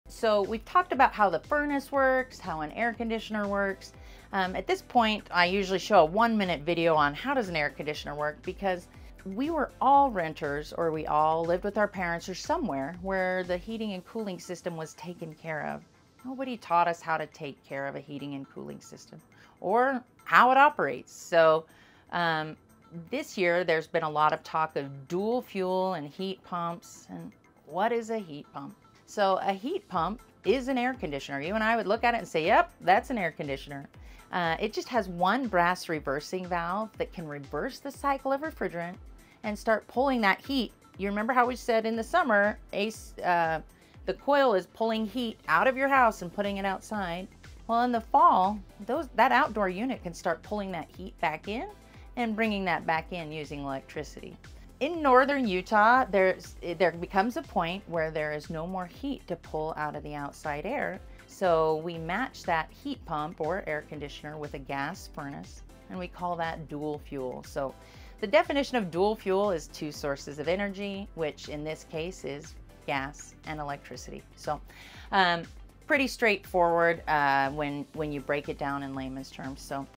Heat Pumps & Dual Fuel
Heat Pump or AC? What’s the Difference?
Most of us grew up in homes where the HVAC system was just there—nobody ever taught us how it works. This quick video breaks it down in simple, easy-to-understand terms!

With all the talk about heat pumps and dual fuel systems this year, here’s the big takeaway:
👉 A heat pump is simply an air conditioner that can reverse the cycle and pull heat into your home using electricity.
👉 When it gets too cold outside here in northern Utah, we pair it with a gas furnace—this combo is called dual fuel, meaning two energy sources working together for maximum efficiency.

Want to know if a heat pump or dual fuel system is right for your home?